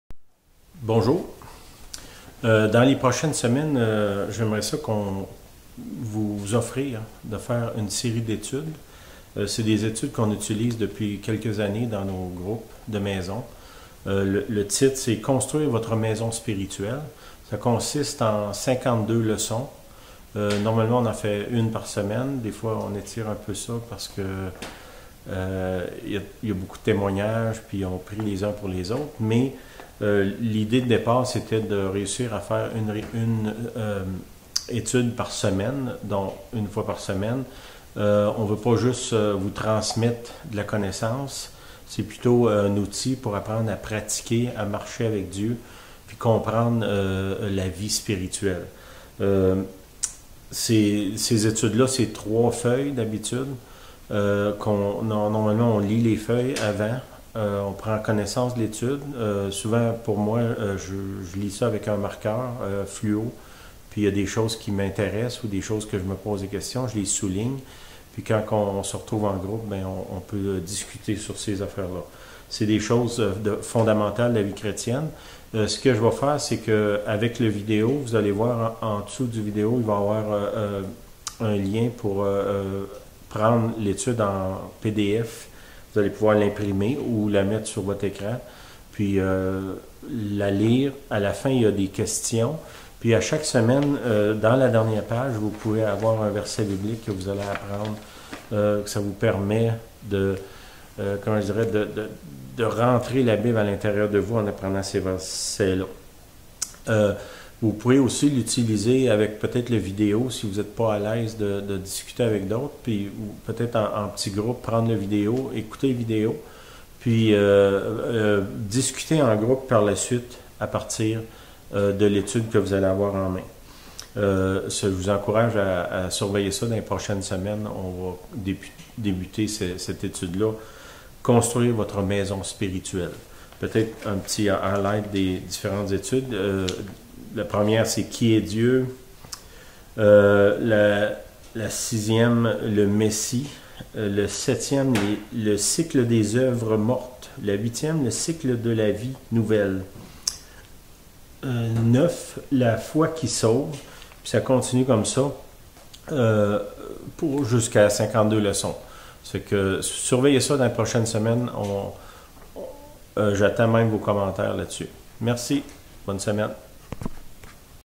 28-02-2013. Introduction à l'étude "Construire une maison spirituelle".
Une étude systématique pour grandir dans la foi et dans la connaissance du Père, du Fils Jésus et du Saint-Esprit.